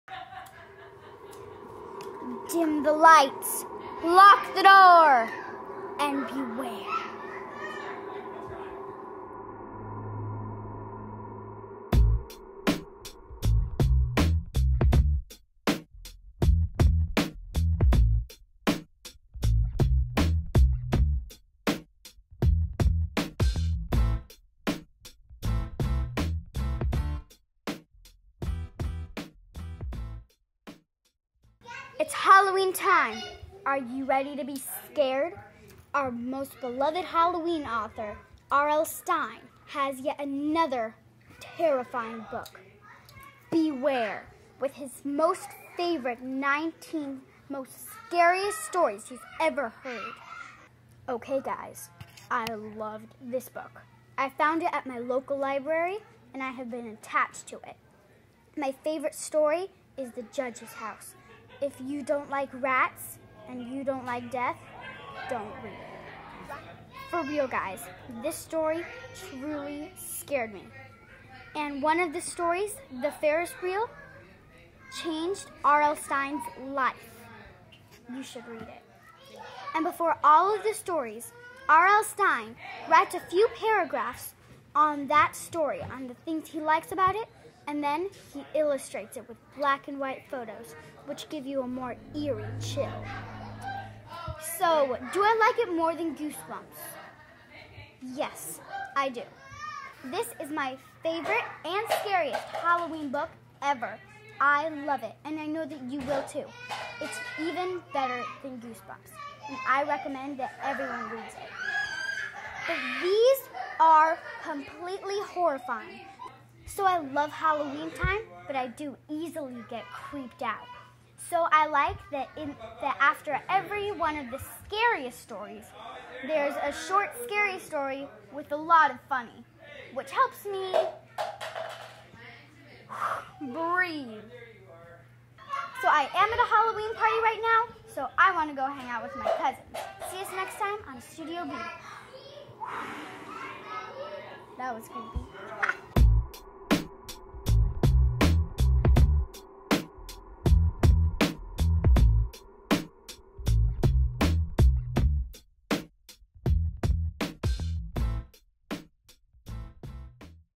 Beware! R.L. Stine
Do you like scary stories? Have you read Goosebumps and your looking for more? Introducing Beware! This is an awesome book with 19 stories retold by RL Stein himself.  My Favorite part is his introduction to each chapter so you can see why he loves the stories.  Enjoy!